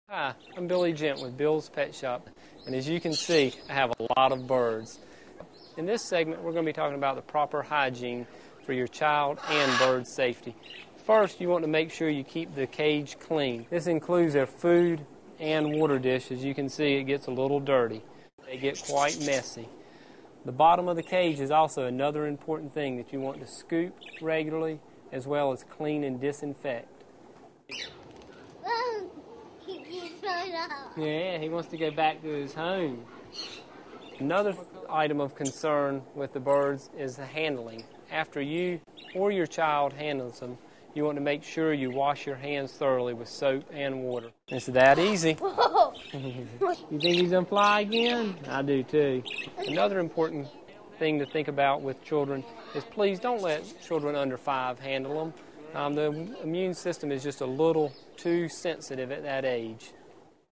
Pet Bird Hygiene for Children
Pet Bird Hygiene for Children. Part of the series: Pet Birds for Children. Proper pet bird hygiene is important for the safety of the child and the bird. Teach a child proper pet bird hygiene and handling with tips from a pet store owner in this free pet video.